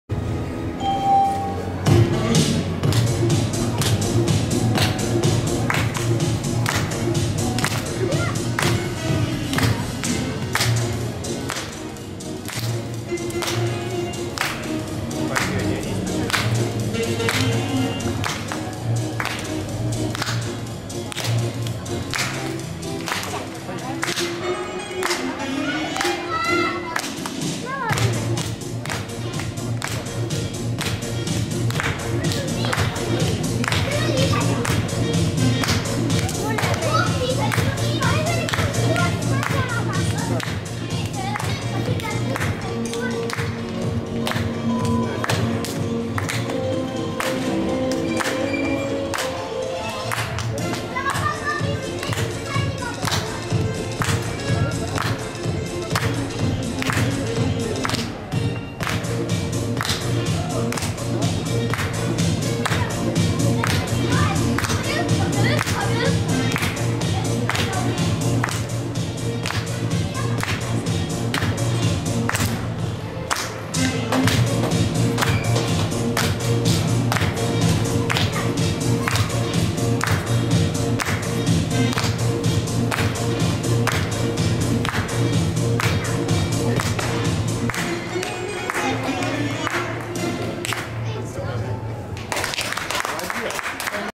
Carmen Marii Aesma rope 2 place Lootuste KV 2012 Tartu etapp
Carmen Marii hüpits 2002a. Tartu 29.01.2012